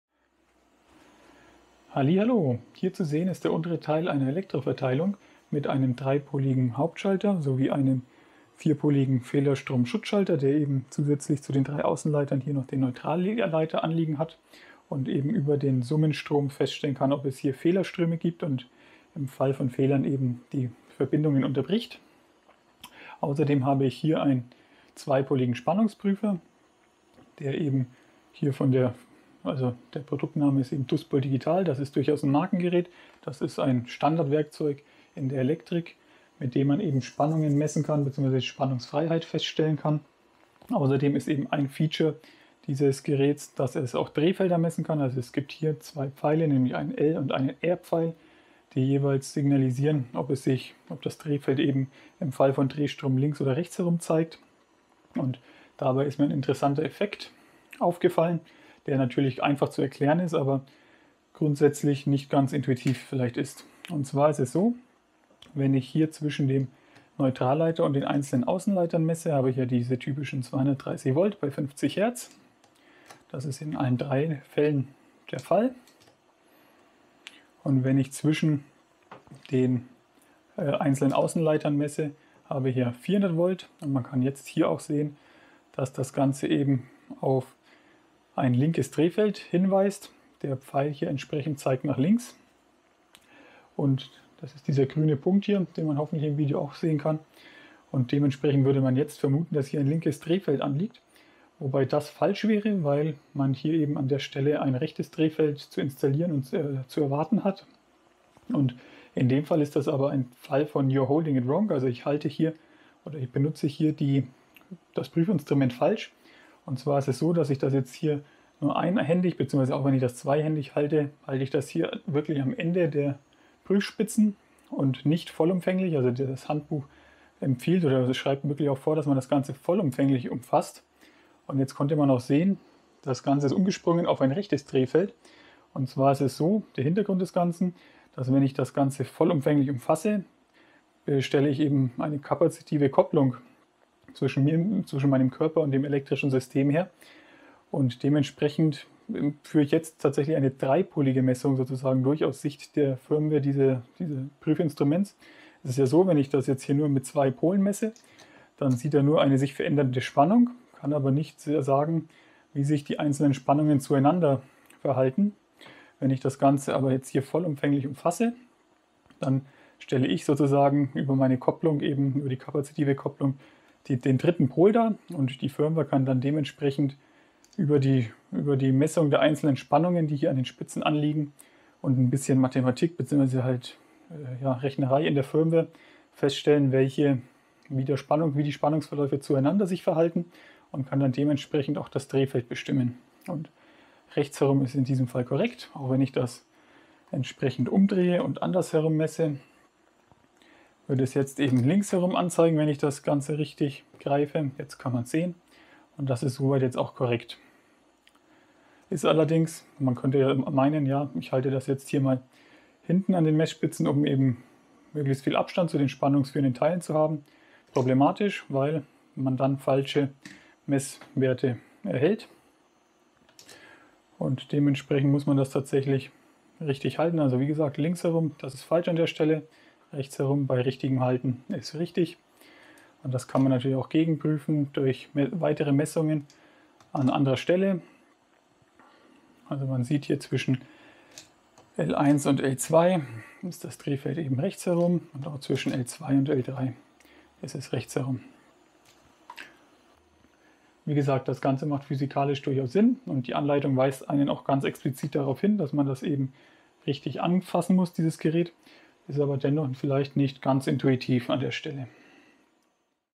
DUSPOL richtig benutzen - Drehfeldmessung
Man kann einen Spannungsprüfer (z.B. DUSPOL) tatsächlich falsch halten. Dies führt zu falschen Messergebnissen (z.B. bei einer Drehfeldmessung).

In diesem Video zeige bzw. erkläre ich diesen Effekt anhand eines Duspol digital von Benning. und zeige wie man diesen richtig benutzt. Auch bei anderen Messungen (z.B. einpolige Phasenbestimmung) kann es zu diesem Effekt kommen. Ähnliche Effekte können z.B. auch bei einfachen Phasenprüfern (Lügenstift) auftreten, wenn man diese falsch bedient/hält.

Meine Wunschliste (Amazon)